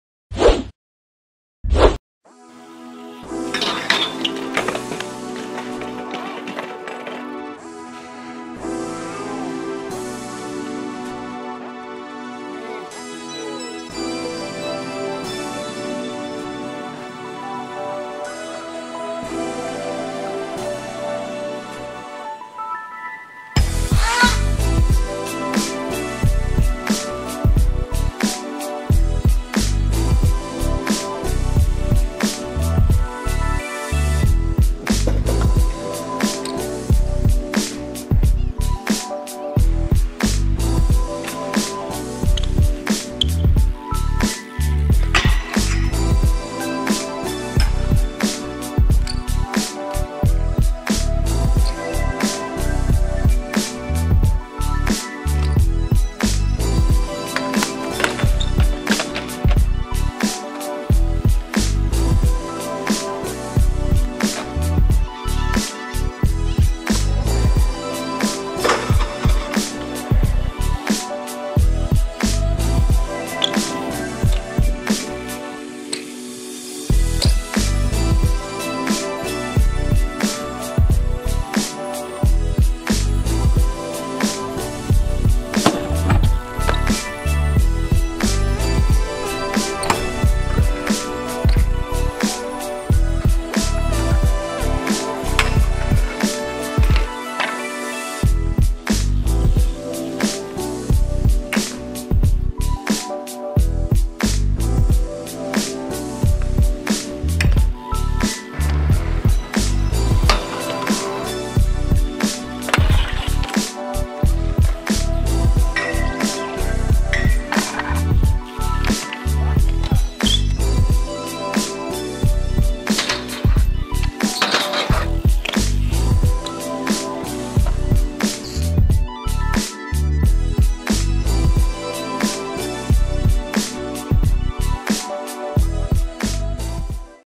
Sessão livre | Caios
Sessão com os riders Caio Alexandre e Caio Henrique, desta vez livre das estrategias de um "game of bike" gravado no vídeo anterior.